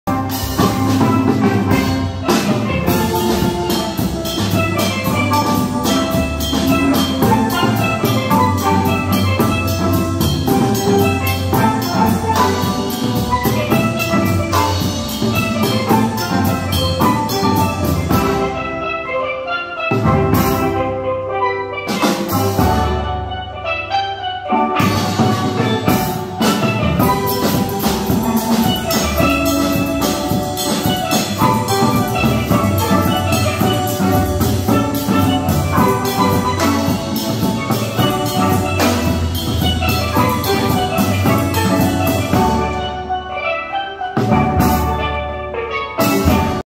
Knights Of Steel at Artis-Naples
TVS Knights of Steel perform at Artis-Naples Community Day - May 21, 2022